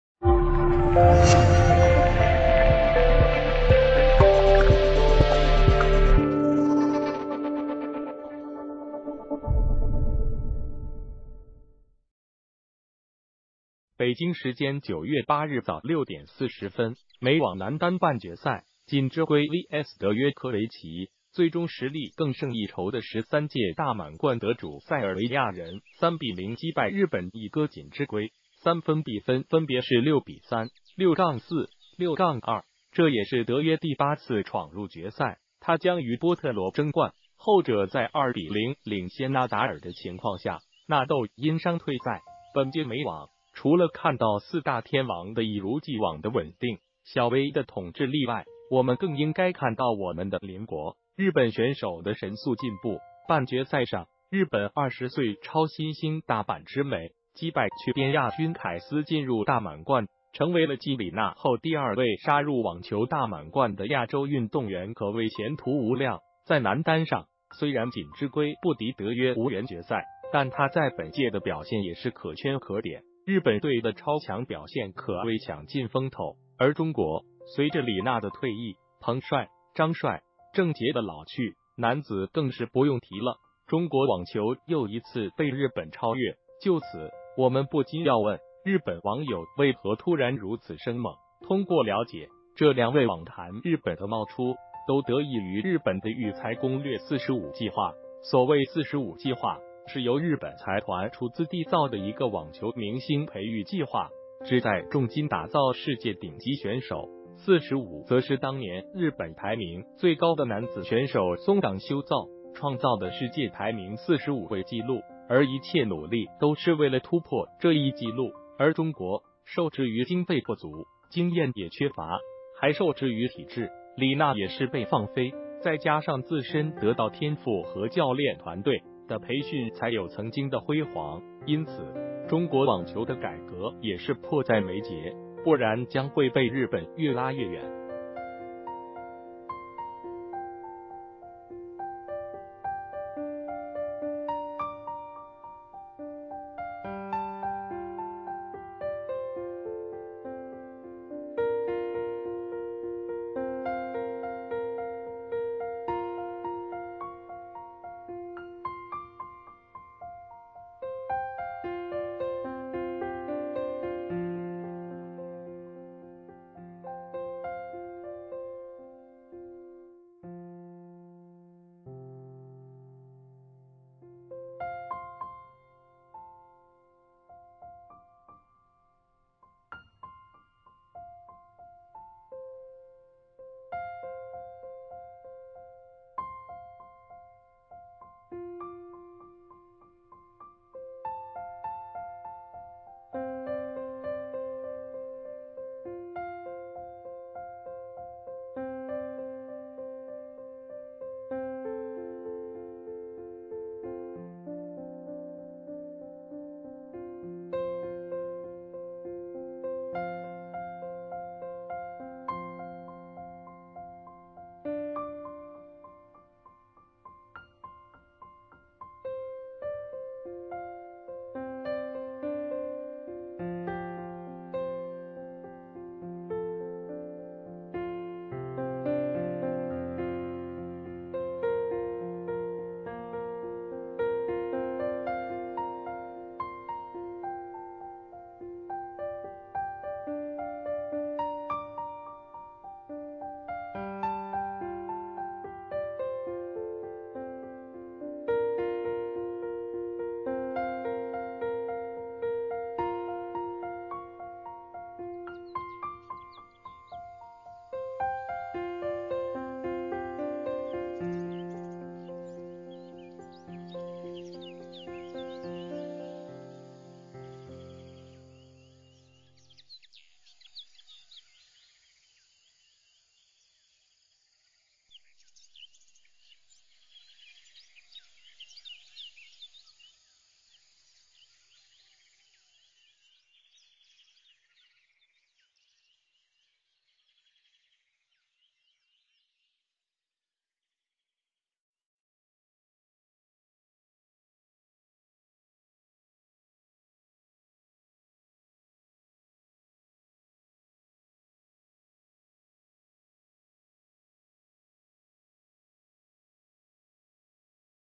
美网大放异彩！日本网球为何突然进步神速！下一个李娜在哪？
谢谢！北京时间9月8日早6点40分，美网男单半决赛，锦织圭vs德约科维奇。最终实力更胜一筹的13届大满贯得主塞尔维亚人3-0击败日本一哥锦织圭。三分比分分别是6-3、6-4、6-2！这也是德约第8次闯入决赛，他将于波特罗争冠，后者在2-0领先纳达尔的情况下，纳豆因伤退赛。本届美网，除了看到四大天王的一如既往的稳定，小薇的统治力外。我们更应该看到我们的邻国，日本选手的神速进步。半决赛上，日本20岁超新星大阪直美击败去边亚军凯斯进入大满贯，成为了继李娜后第二位杀入网球大满贯的亚洲运动员可谓前途无量。在男单上，虽然锦织圭不敌德约无缘决赛，但他在本届的表现也是可圈可点，日本队的超强表现可谓抢尽风头。而中国，随着李娜的退役，彭帅，张帅、郑洁的老去，男子更是不用提了，中国网球又一次被日本超越。​就此，我们不禁要问，日本网友为何突然如此生猛？通过了解，这两位网坛日本的冒出都得益于日本的育才攻略“45计划”。所谓“45计划”是由日本财团出资缔造的一个网球明星培育计划，旨在重金打造世界顶级选手，“45”则是当年日本排名最高的男子选手松岗修造创造的世界排名45位纪录，而一切努力都是为了突破这一纪录。而中国，受制于经费不足，经验也缺乏，还受制于体制。李娜也是被“放飞”，在加上自身得到天赋和教练团队的培训才有曾经的辉煌。因此，中国网球的改革也是迫在眉睫，不然将会被日本越拉越远。